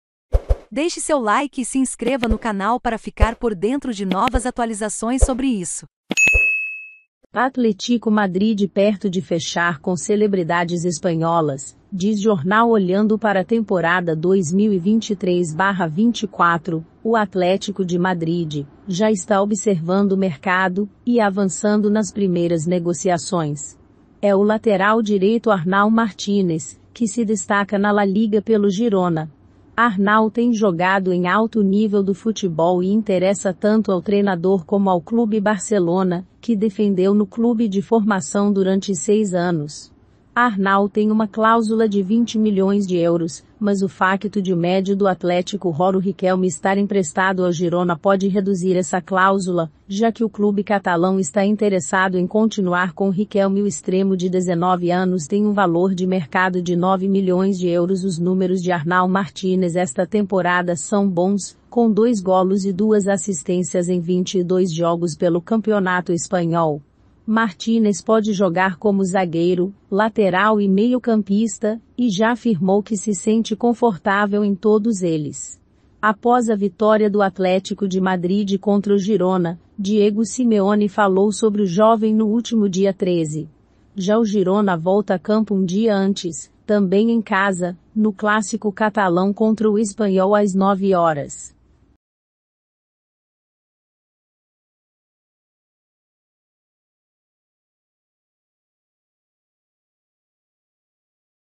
ATLETICO MADRID PERTO DE FECHAR COM CELEBRIDADES ESPANHOLAS. DIZ JORNAL
Olhando para a temporada 2023/24. o Atlético de Madrid já está de olho no mercado e avançando nas primeiras negociações. Este é o lateral direito Arnau Martínez. que se destaca na La Liga pelo Girona.